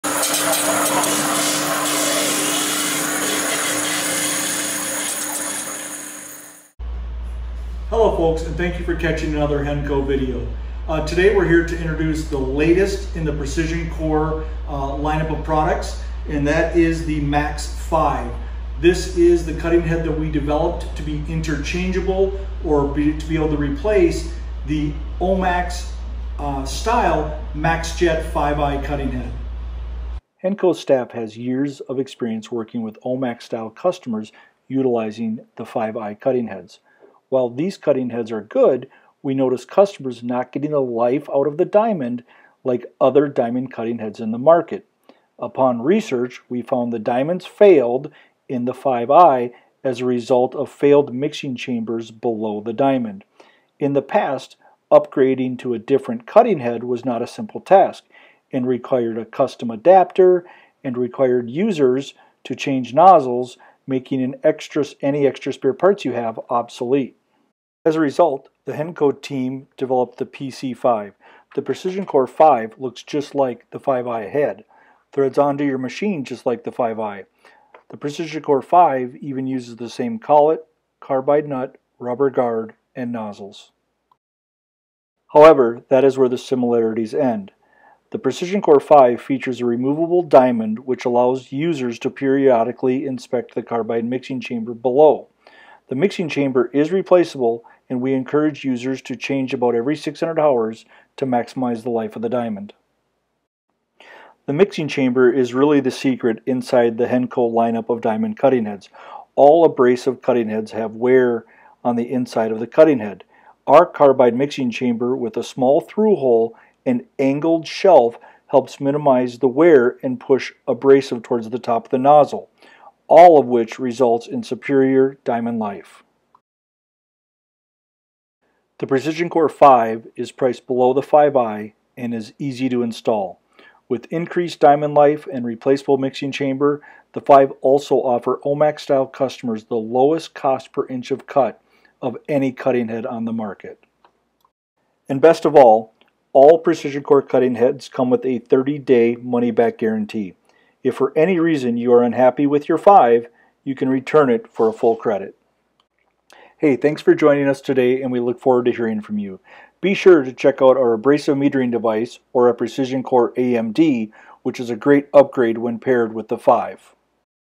Hennco PrecisionCore Five for OMAX Nozzle Assembly
The PrecisionCore Five was specifically engineered to upgrade and replace your OMAX Maxjet 5 or Maxjet 5i nozzle assembly.  The FIVE external geometry is designed to mirror the 5i while utilizing the leading edge technology of our PrecisionCore diamond cutting heads on the inside.